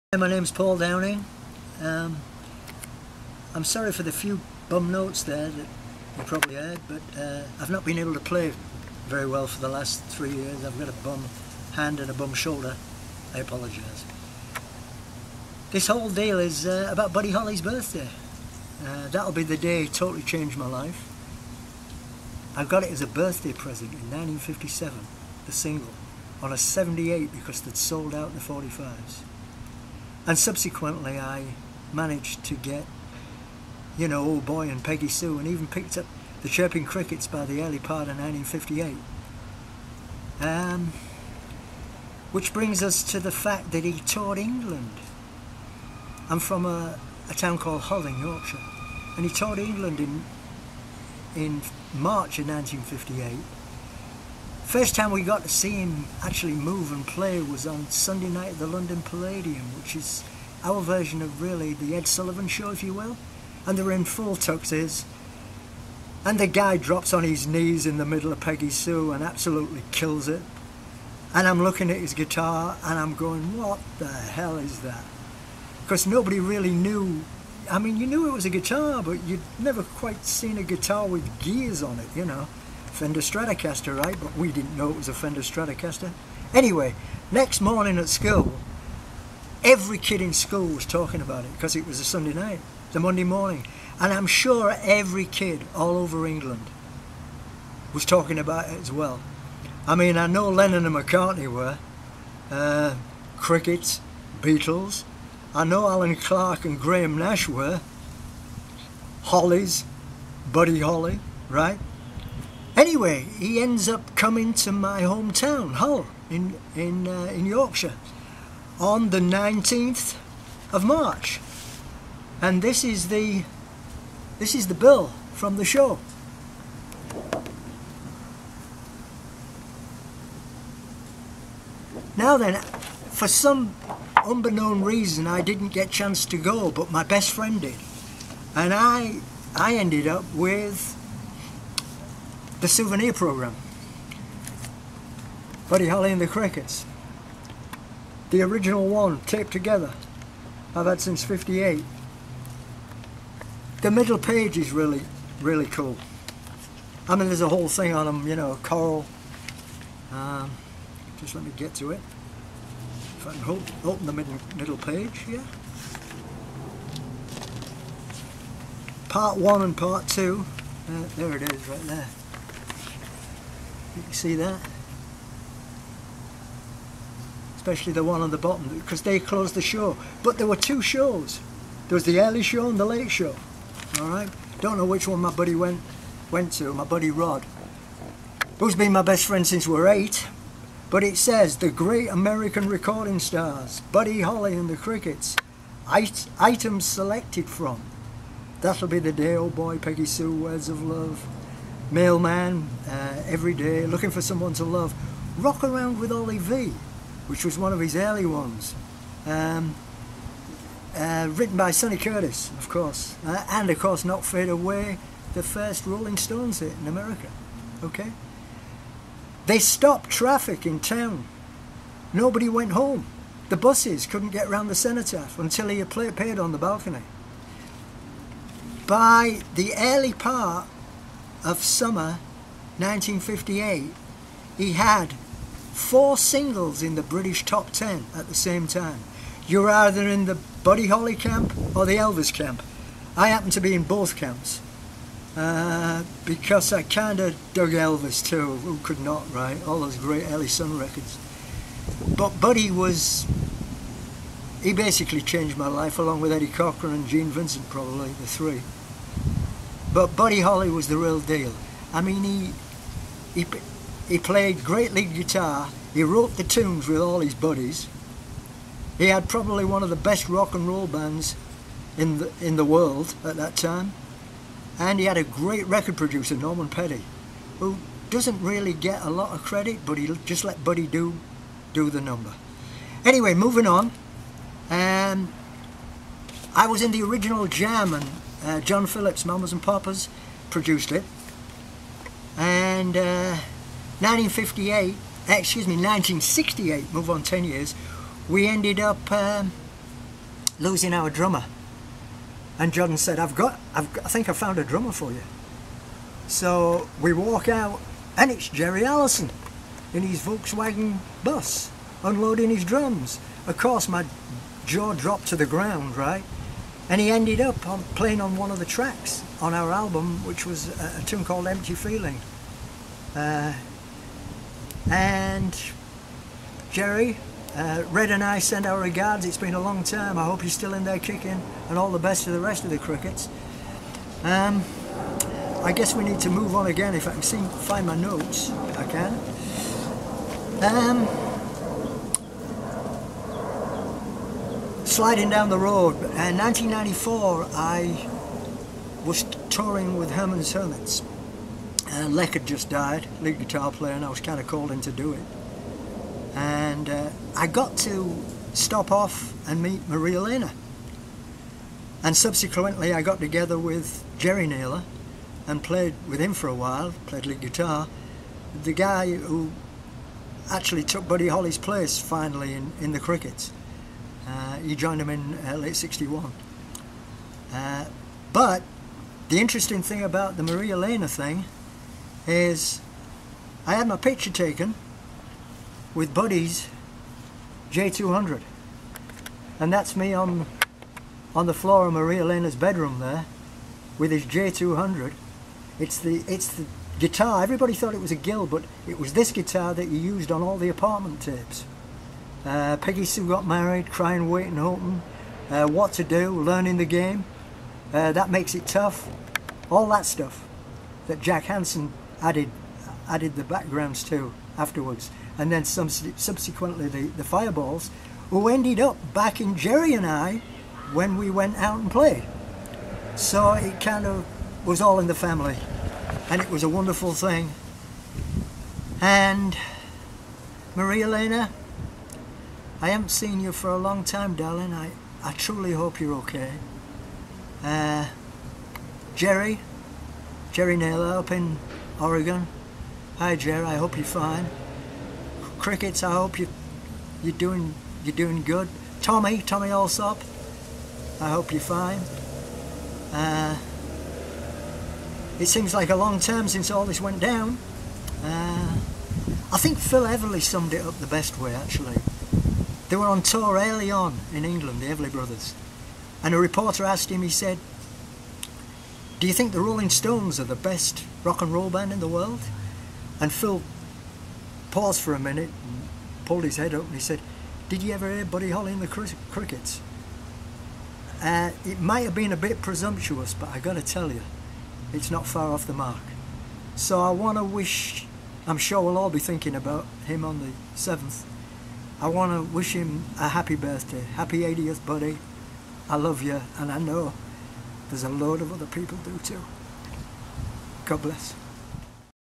Paul Downing's History of Buddy Holly
“Rock roots” don’t go much deeper than those of Paul Downing, who played guitar in the band that backed the original The Mamas & The Papas, toured with Herman’s Hermits, and played in a recent incarnation of The Standells. Paul talks about how Buddy Holly Influenced him.